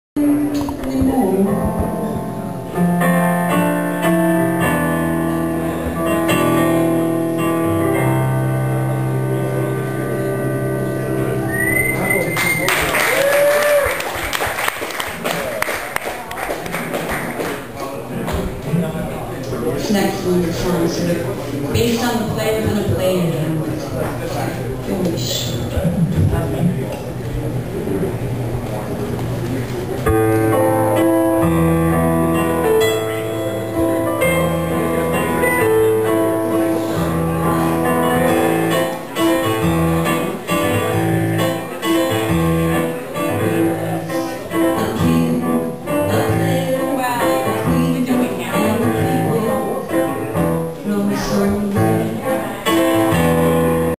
Stacy Rock - Laila's Lounge
Stacy Rock performing solo at Laila's Lounge in Williamsburg - Brooklyn, NY 10/04/06